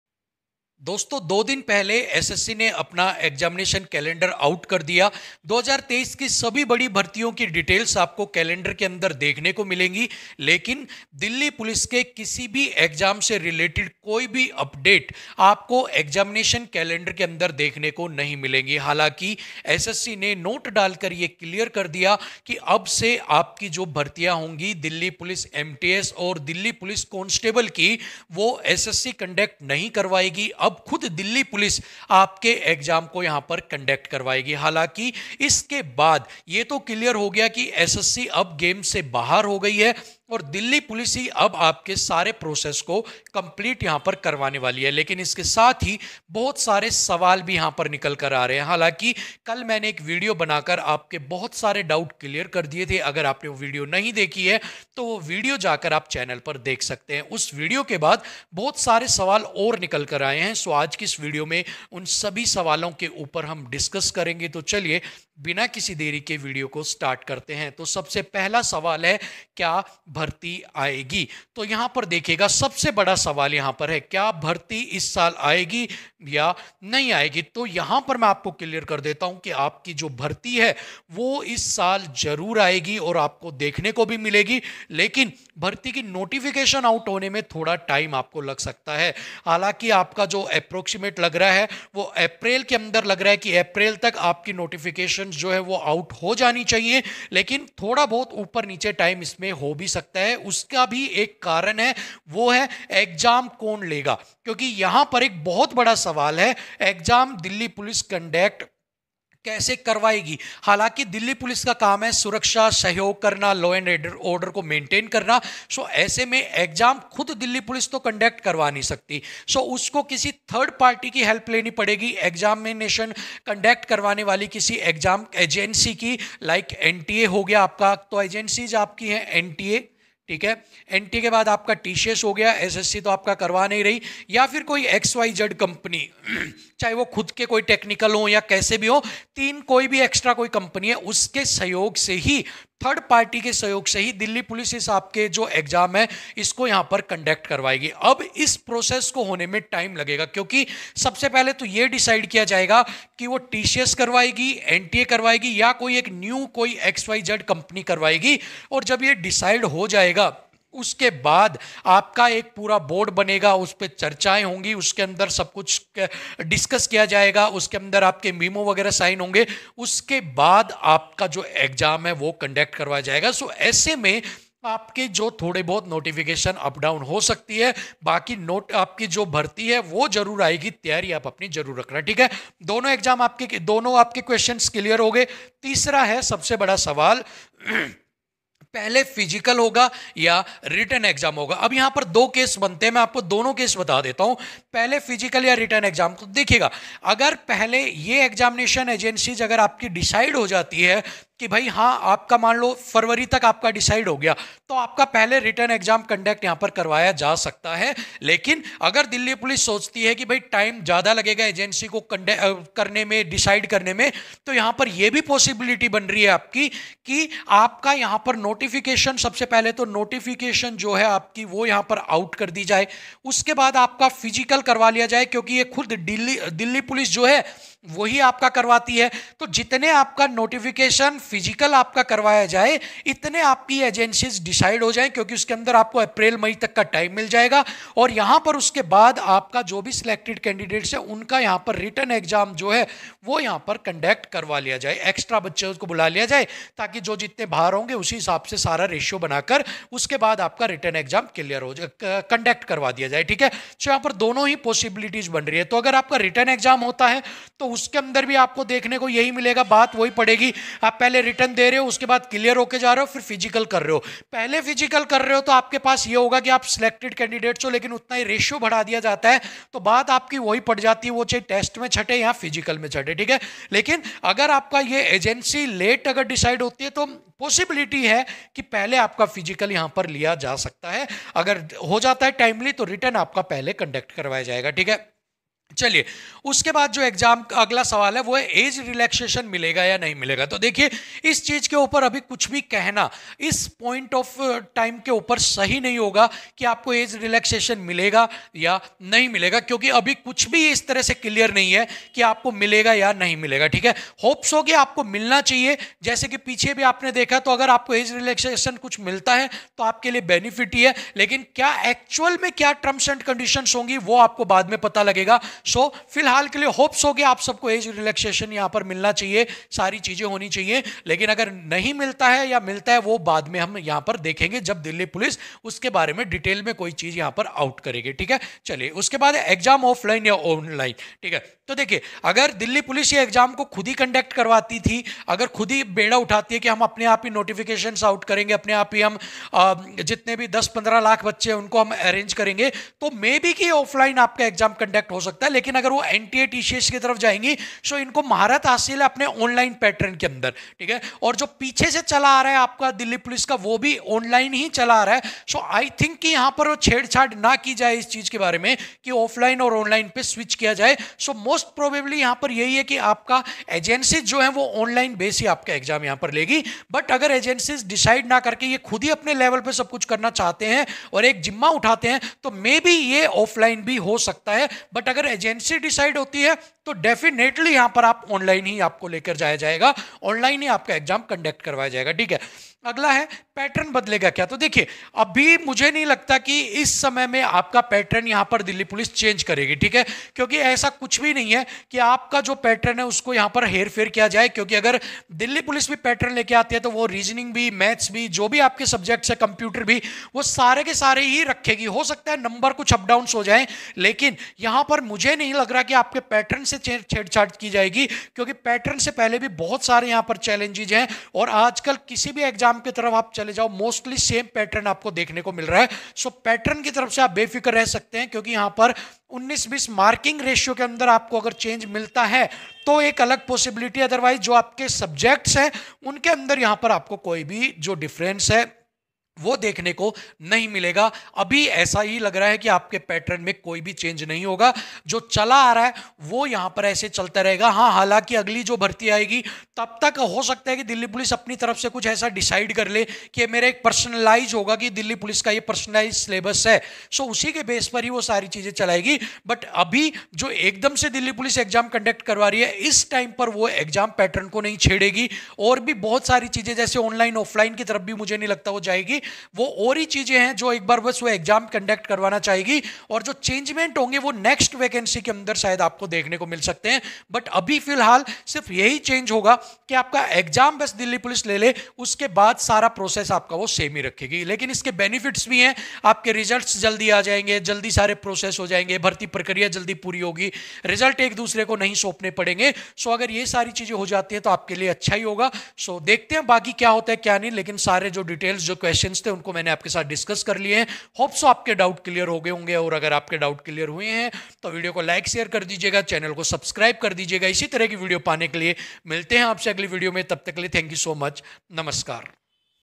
delhi police constable new vacancy 2023 latest update | दिल्ली पुलिस कांस्टेबल latest update today
hi everyone in this video we are going to talk about  Delhi police constable new vacancy 2023 latest update. we will also talk about delhi police constable vacancy 2023 kab aayegi.  some other questions are also answered in this video, so watch the video till the end.

delhi police 490 candidates,
delhi police vacancy 2023,
delhi police bharti 2023,

THANKYOU SO MUCH FOR WATCHING THE VIDEO.
fun learn by anuj